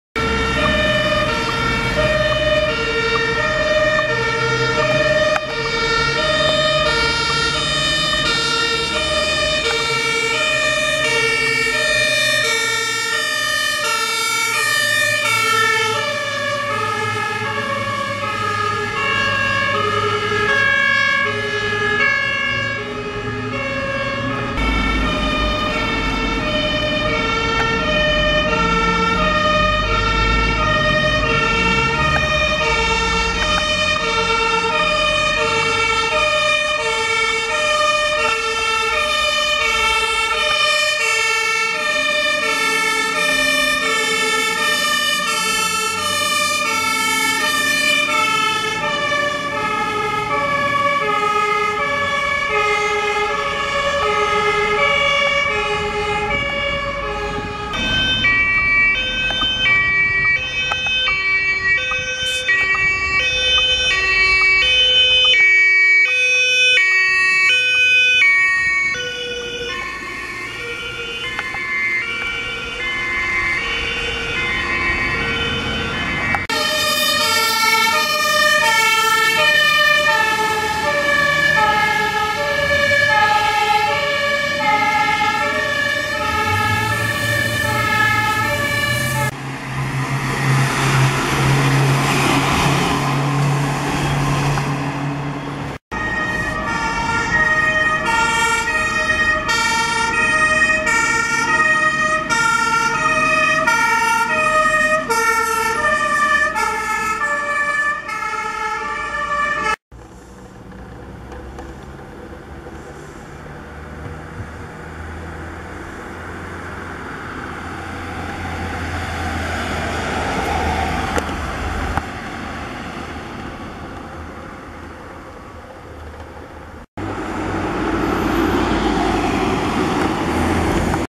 RW 1 + LF 16/12 + ELW 1 + TSF-W + KTLF + 2 KLF-TH FF Greiz
In diesen Video ist von der Freiwilligen Feuerwehr Greiz der Rüstwagen 1 (FLO GRZ 71), das Löschgruppenfahrzeug 16/12 (FLO GRZ 44), der Einsatzleitwagen 1 (FLO GRZ 11/1), von der Ortswehr Obergrochlitz das Tragkraftspritzenfahrzeug-Wasser (FLO Obergrochlitz 41), von der Ortswehr Gommla das Kleintanklöschfahrzeug (FLO Gommla 41) und von den Ortswehren Kurtschau und Moschwitz jeweils 1 Kleinlöschfahrzeug-Thüringen.

gefilmt wurde im Oktober 2019
mit einer Canon PowerShot SX50HS